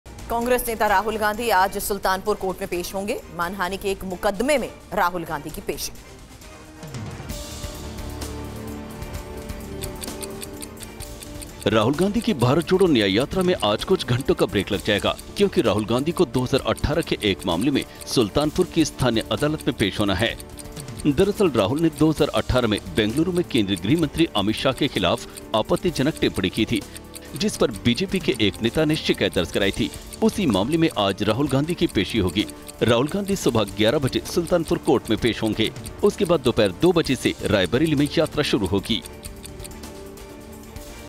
Rahul Gandhi की आज कोर्ट में पेशी, न्याय यात्रा पर लगेगा ब्रेक, जानें क्या है पूरा मामला | Congress
राहुल गांधी की भारत जोड़ो न्याय यात्रा में आज कुछ घंटों का ब्रेक लग जाएगा...क्योंकि राहुल गांधी को 2018 के एक मामले में सुल्तानपुर की स्थानीय अदालत में पेश होना है... दरअसल राहुल ने 2018 में बेंगलुरु में केंद्रीय गृहमंत्री अमित शाह के खिलाफ आपत्तिजिनक टिप्पणी की थी...जिसपर बीजेपी के एक नेता ने शिकायत दर्ज कराई थी...उसी मामले में आज राहुल गांधी की पेशी होगी...